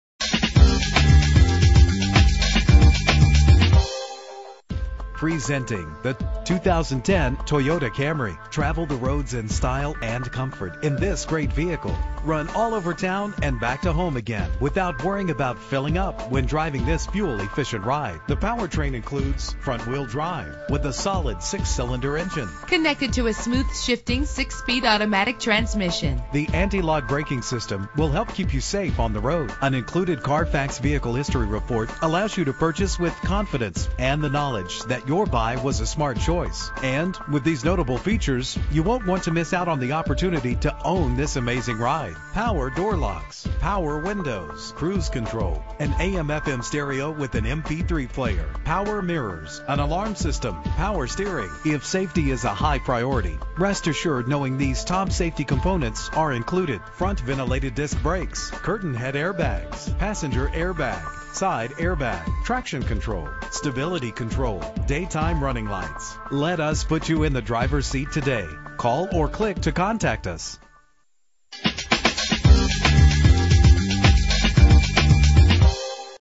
2010 Toyota Camry - Avenel New Jersey
Year: 2010
Make: Toyota
Model: Camry
Trim: LE
Engine: 3.5 liter 6 cylinder 24 valve
Transmission: 6-Speed Automatic
Color: Black
Mileage: 45806
Address:
90-100 Route 1 North